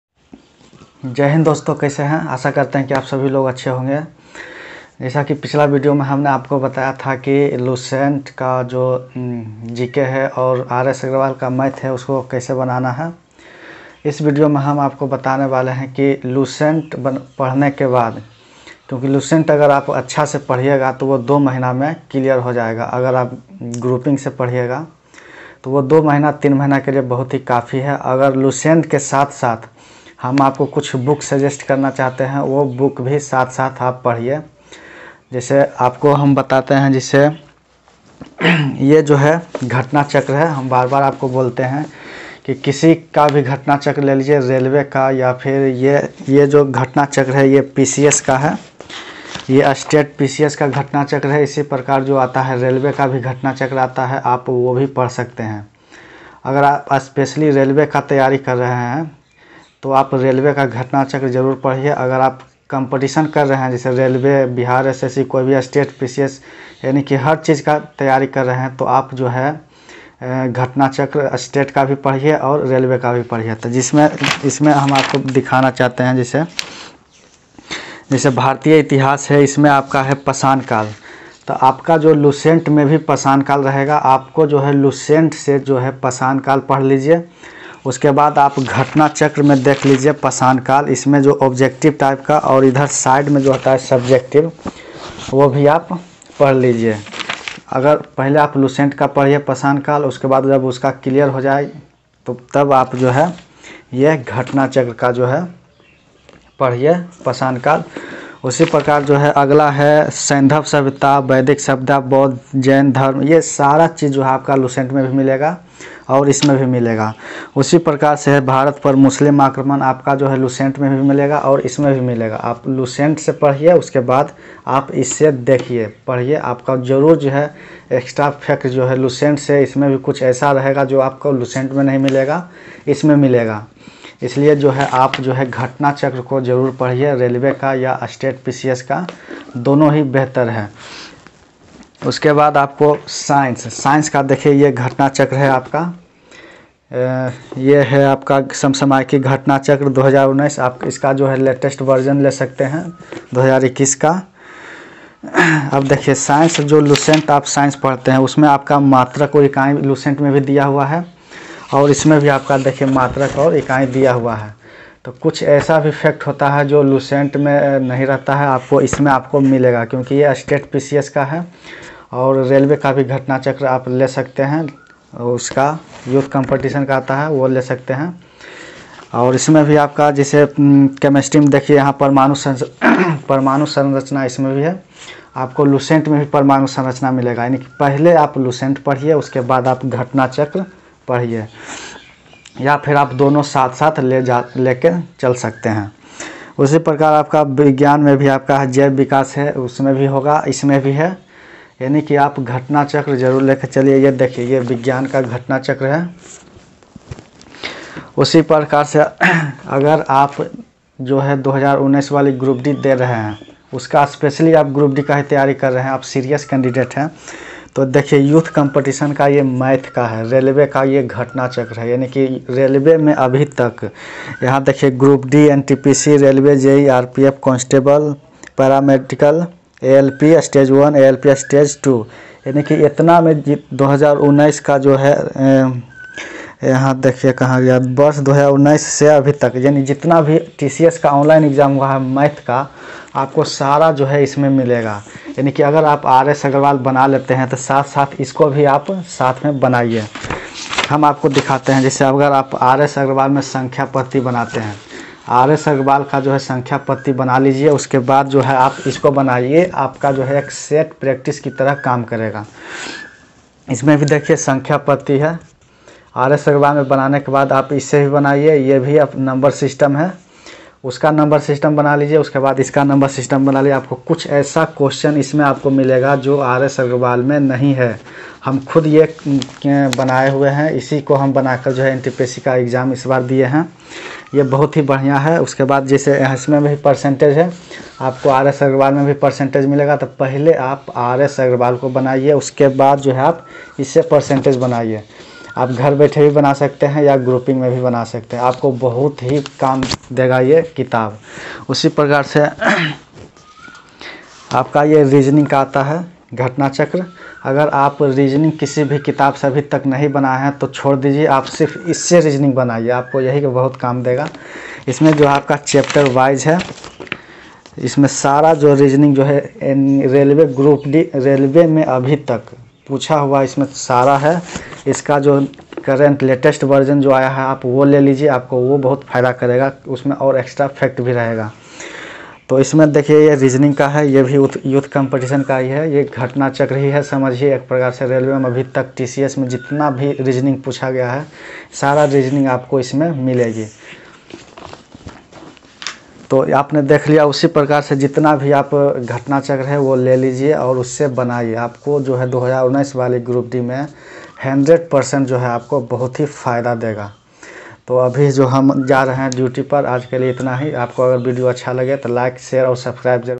GK और GS में एक्स्ट्रा fact के लिए ये किताबें पढ़े।। target Railway group d 2019
is video me humne bataya hai ki lucent ke alawa science aur gs ke extra fact ke liye kaun si kitabe achhi hai